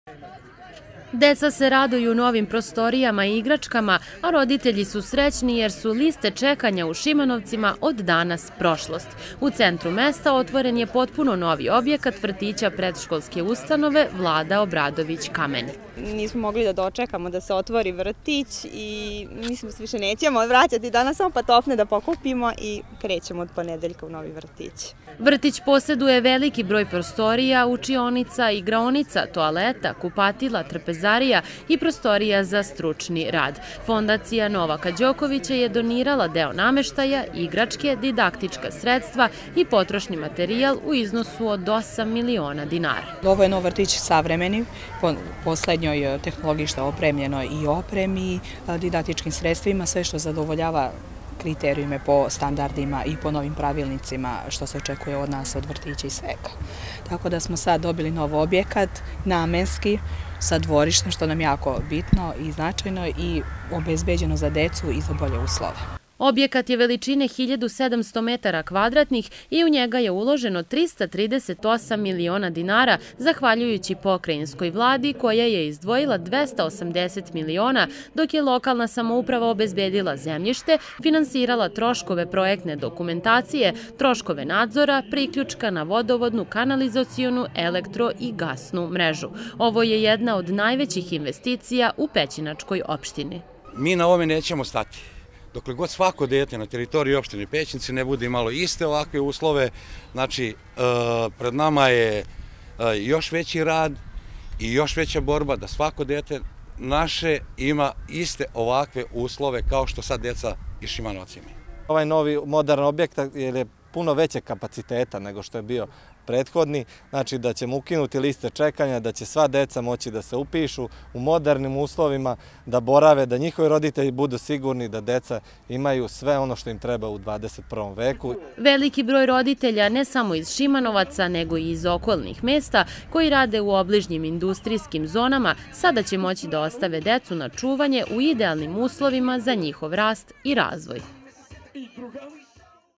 U Šimanovcima otvorena nova zgrada vrtića
Danas je u Šimanovcima otvorena nova zgrada vrtića. Na više od 1700 kvadratnih metara deca i vaspitači će na raspolaganju imati sve ono što imaju deca u razvijenim evropskim gradovima.